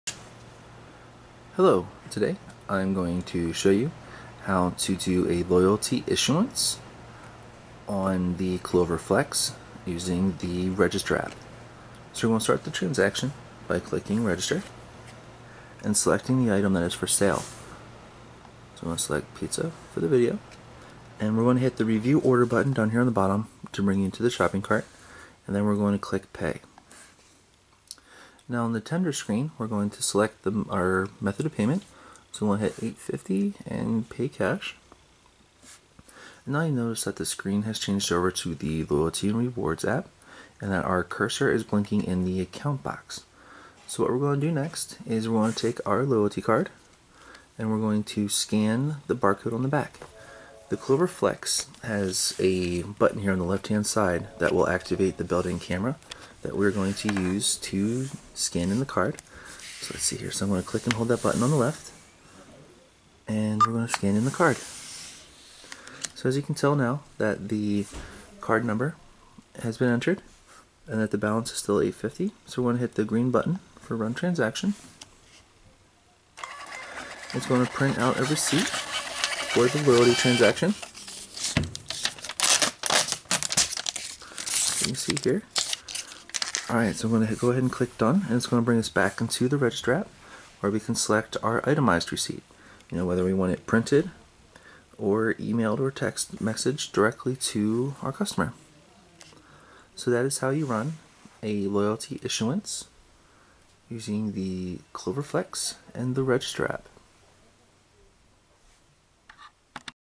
How to Issue Loyalty Points on the Clover Flex through the Register App
This video will demonstrate how to issue loyalty points through the register app on your clover flex.